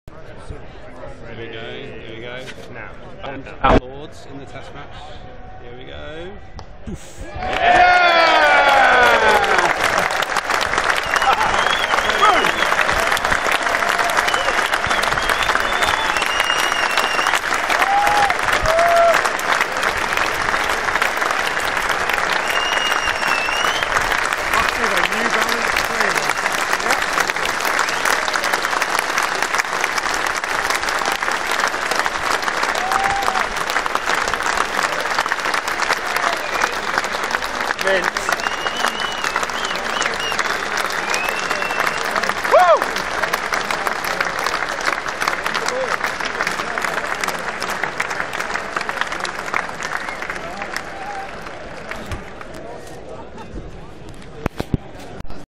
Gary Ballance makes a hundred, July 18th 2014
Second day of the second test at Lord's. Ballance flicks Binny to the leg side boundary to bring up his century. England lost the match but won the series 3-1.